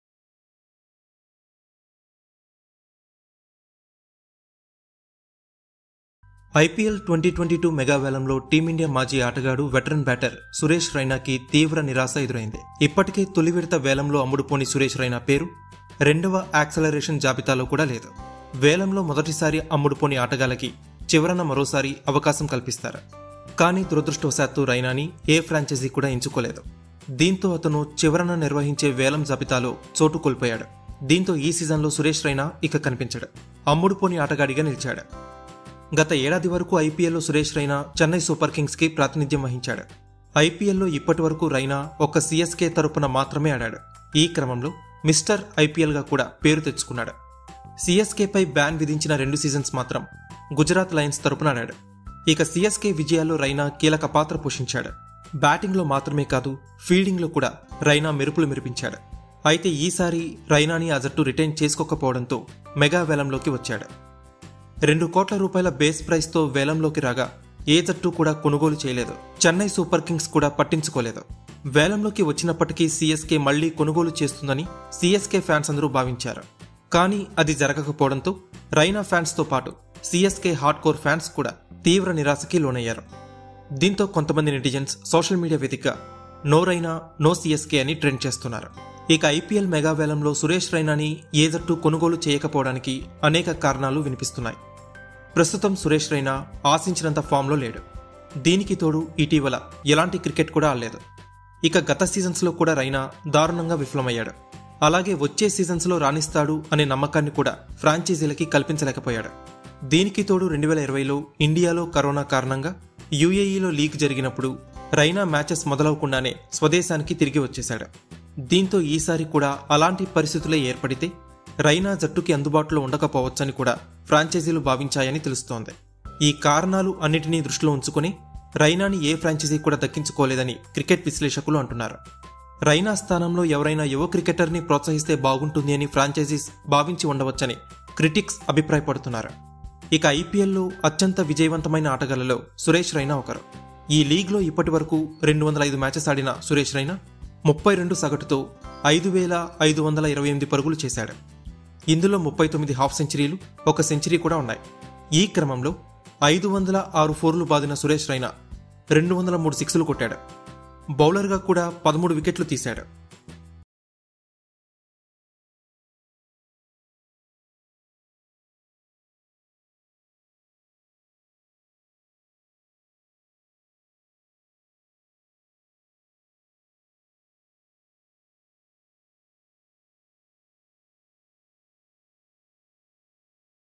IPL 2022 : Why CSK Too Ignored Chinna Thala Suresh Raina? No Raina No CSK | Oneindia Telugu
IPL Auction 2022 : Suresh Raina Goes Unsold In IPL 2022 Mega Auction

ఐపీఎల్ 2022 మెగా వేలంలో టీమిండియా మాజీ ఆట‌గాడు, వెట‌ర‌న్ బ్యాట‌ర్ సురేష్ రైనాకు తీవ్ర నిరాశ ఎదురైంది. ఇప్ప‌టికే తొలి విడ‌త వేలంలో అమ్ముడుపోని సురేష్ రైనా పేరు రెండవ యాక్సిలరేషన్ జాబితాలో కూడా లేదు. వేలంలో మొద‌టి సారి అమ్ముడు పోని ఆట‌గాళ్ల‌కు చివ‌ర‌న మ‌రోసారి అవ‌కాశం ఇస్తారు